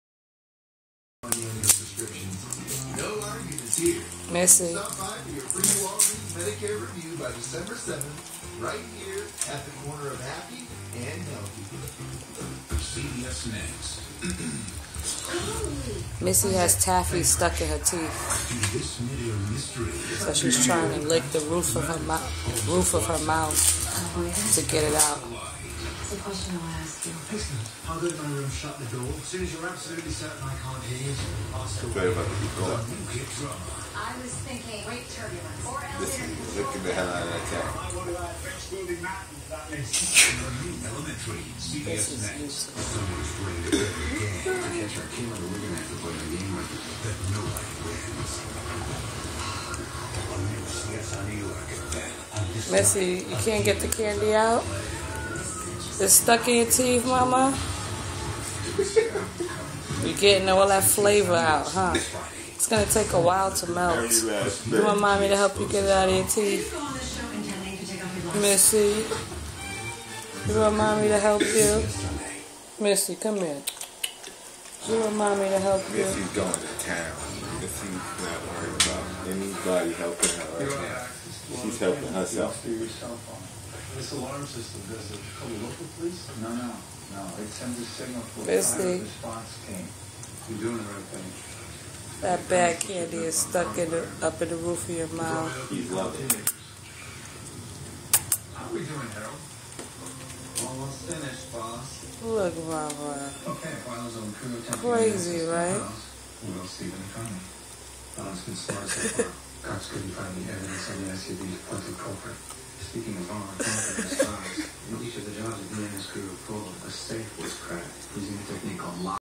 Brooklyn Beagle Goes Crazy for Taffy
A beagle licking Taffy off the roof of her mouth.